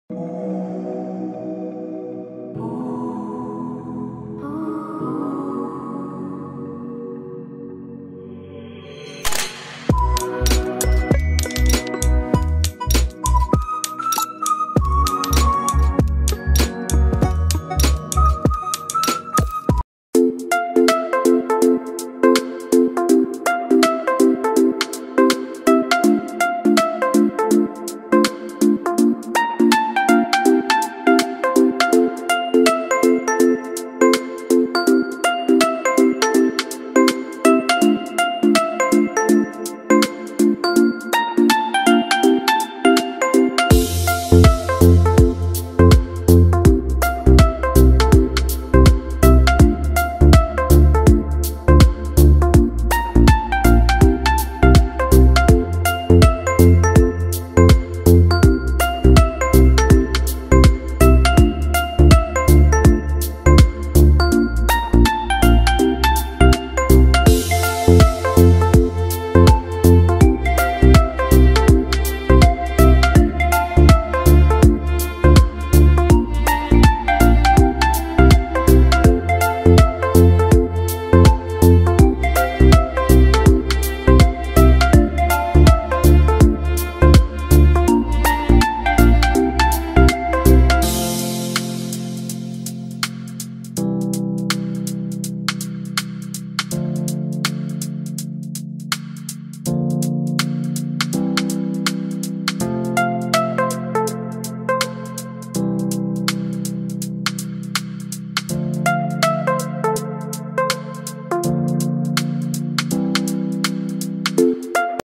Identifying species of hamster!!!
This video is about species of hamster. If you are a hamster owner you should know what species your hamster is and where they come from their length and more! i hope this video helped anyone who maybe doesn't know all the hamster species thanks for watching the video see you in the next video bye!